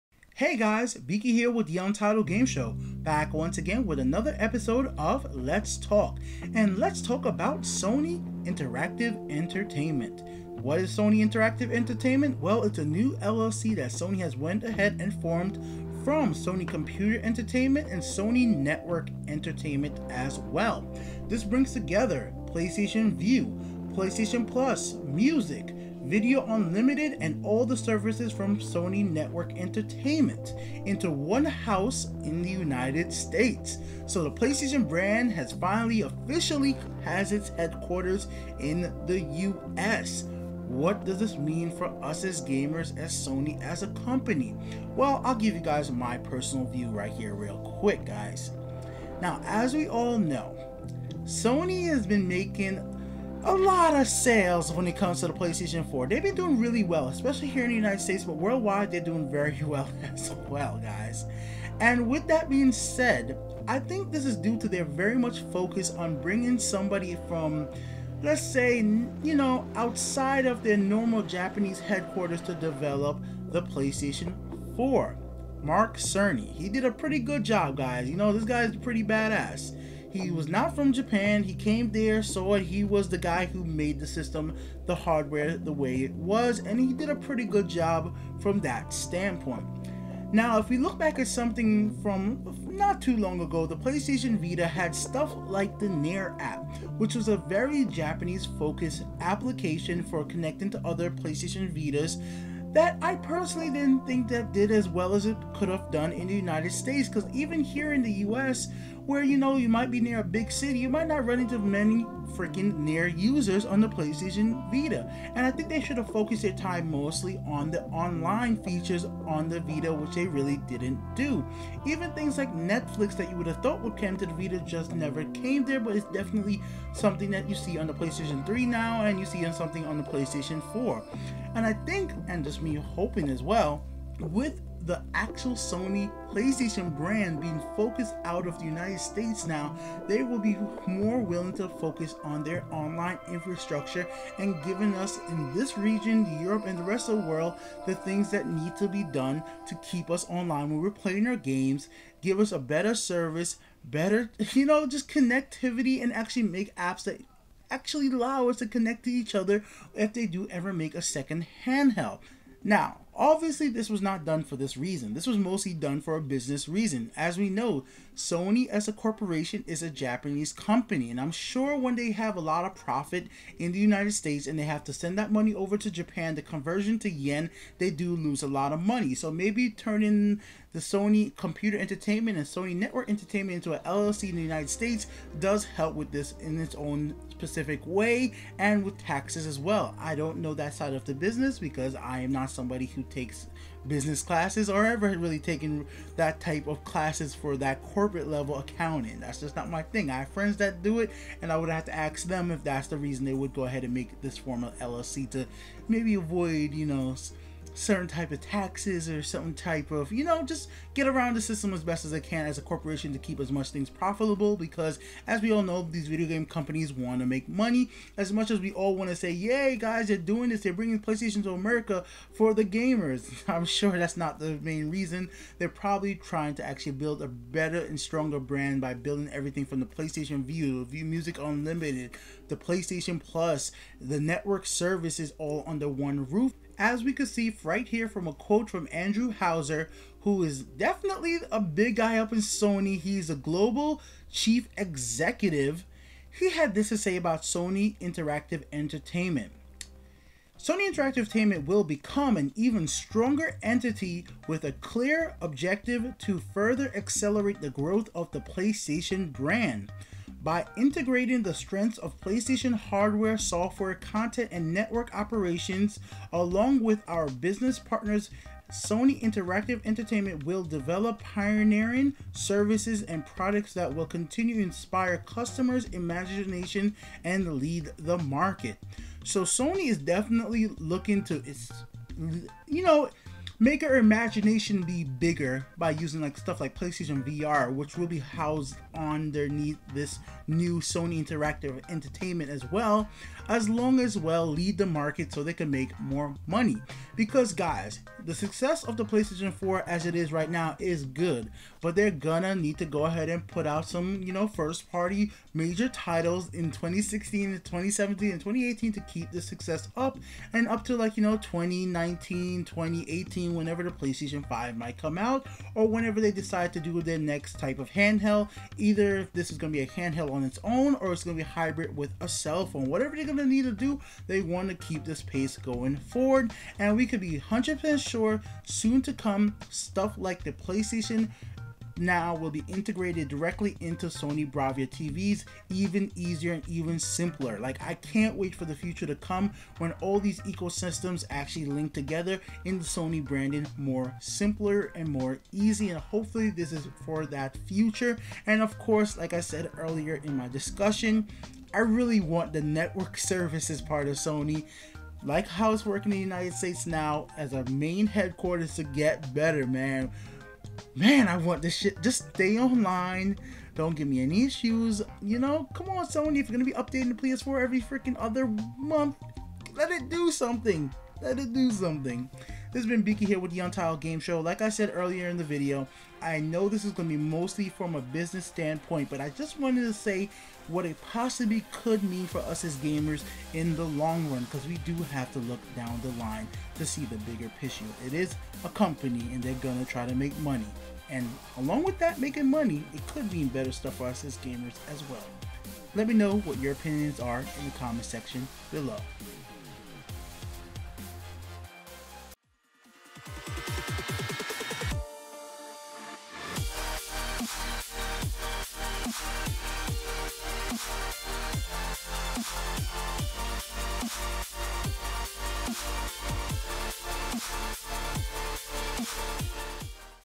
Let's Talk Sony Computer Entertainment Becoming Sony Interactive Entertainment
This weeks "Let's Talk" is about Sony consolidating Sony Computer Entertainment (SCE) and Sony Network Entertainment (SNE) to  form Sony Interactive Entertainment LLC (SIE).

Shawn Layden PS Blog Post about SIE:
"Today I’m pleased to share an important new moment in PlayStation history that will power the next 20 years of PlayStation innovation.

We are announcing the formation of a new company called Sony Interactive Entertainment LLC (SIE) that will serve as the headquarters for everything PlayStation. This new company, headquartered out of the United States in San Mateo, California, joins the forces of both Sony Computer Entertainment Inc. and Sony Network Entertainment LLC, combining the strengths and talents of both companies.

The formation of SIE will create an even stronger PlayStation. That means an even bigger emphasis on developing pioneering platforms like PS4 and PlayStation VR, world-class games from our Worldwide Studios development teams, and innovative network services such as PlayStation Vue.

It also means a renewed commitment to making PlayStation the best place to play."